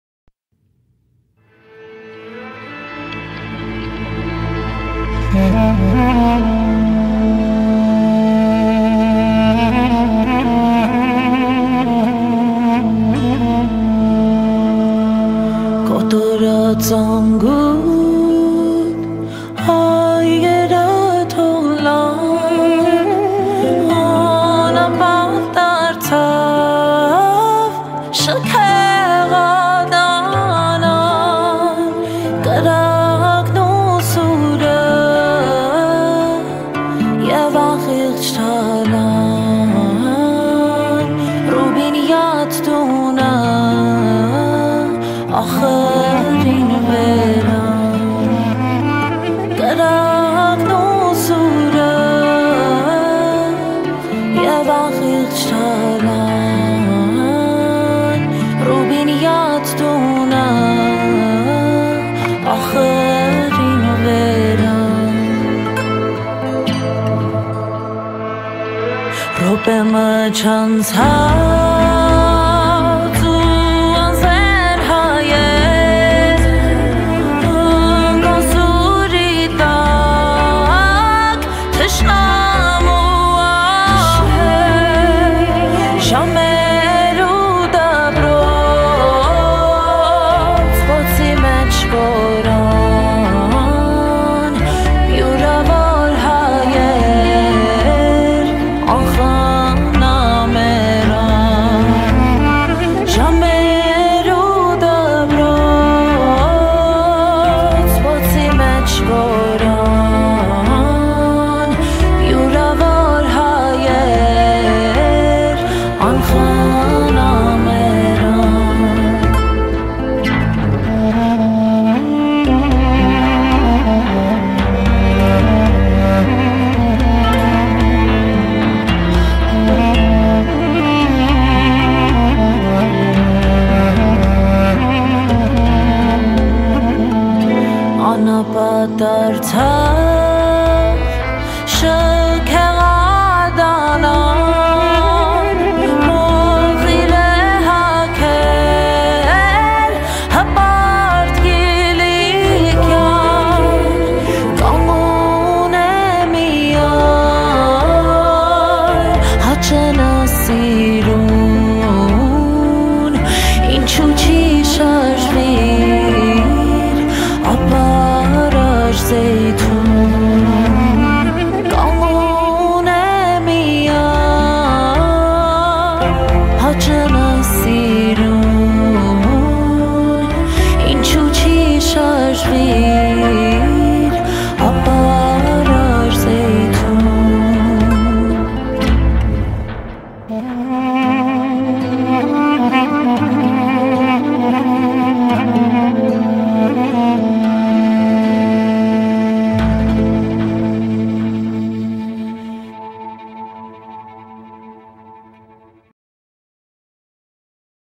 Armenian Genocide memorial cross has been placed underwater in Lebanon
This Video is property of elnashra.com